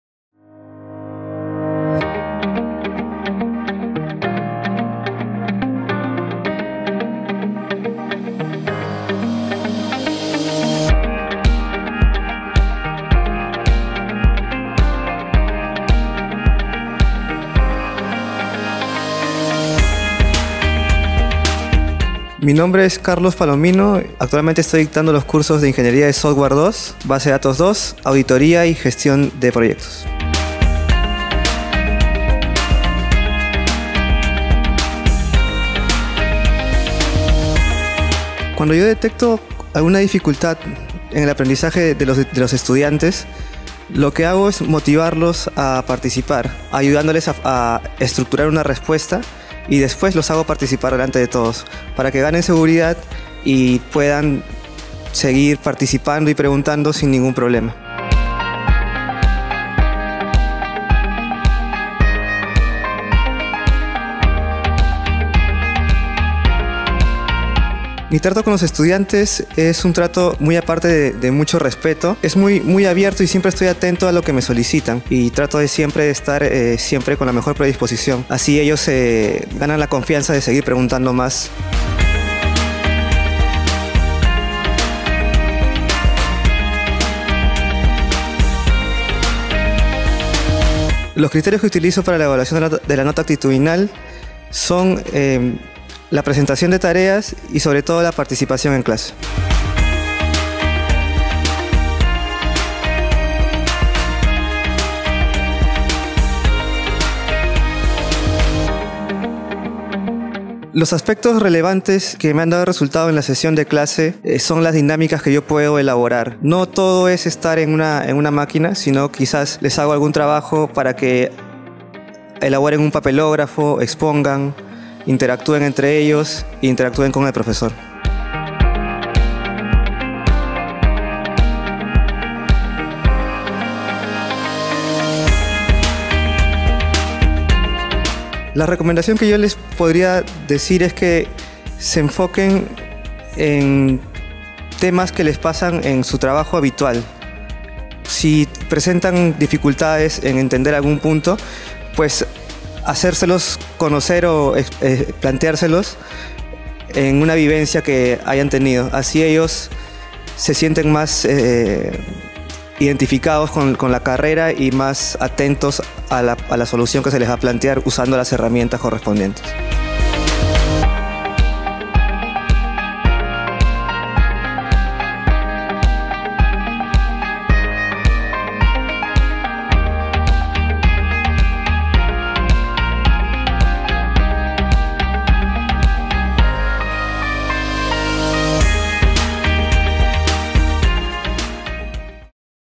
Entrevista al docente: Carlos Palomino
La Escuela Profesional de Ingeniería de Sistemas e Informática de la Universidad de Ciencias y Humanidades, brindó un reconocimiento al profesor Carlos Palomino por la: "Sociabilización de buenas prácticas y experiencias pedagógicas en ingeniería".